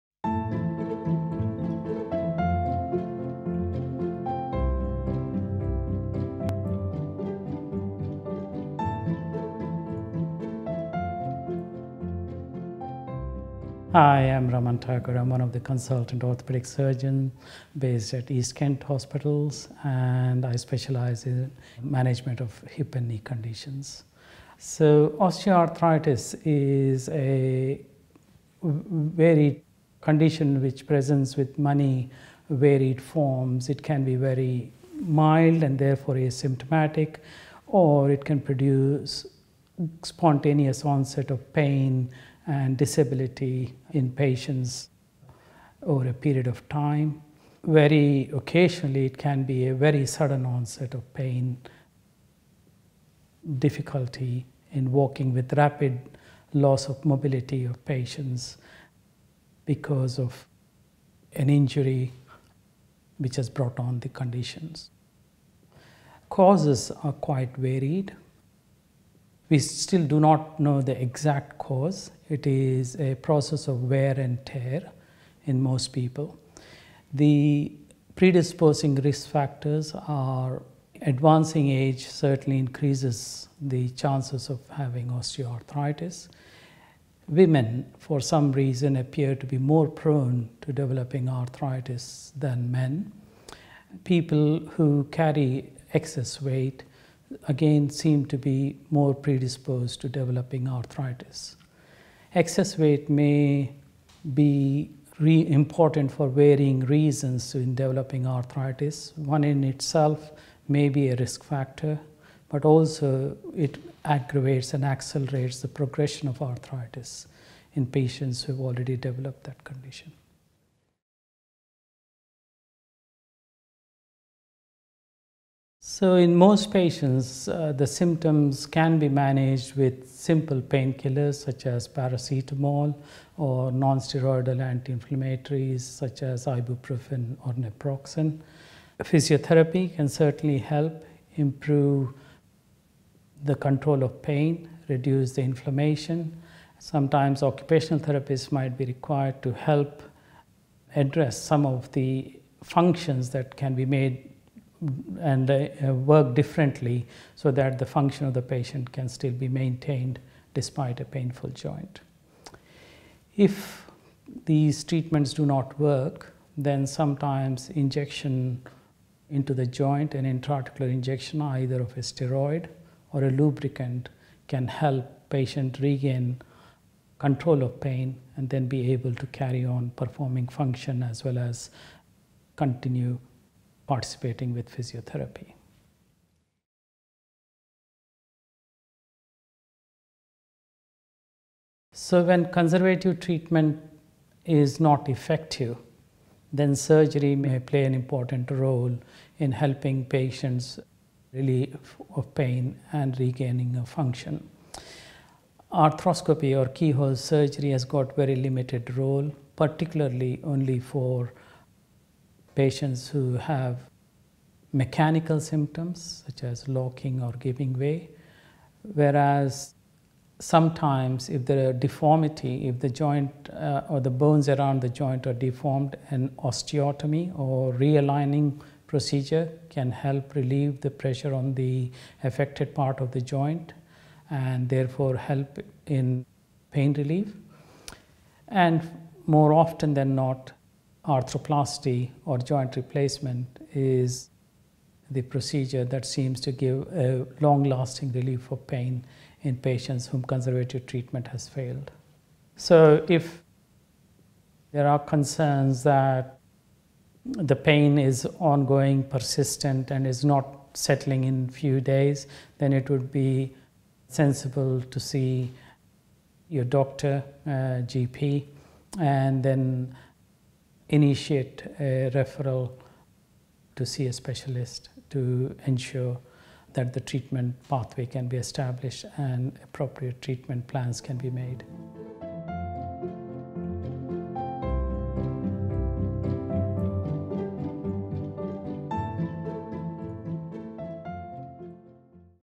Mr Raman Thakur, Consultant Orthopaedic Surgeon at One Ashford Hospital
Mr Raman Thakur, Consultant Orthopaedic Surgeon at One Ashford Hospital discusses hip and knee osteoarthritis, common symptoms and causes, and recommended treatment options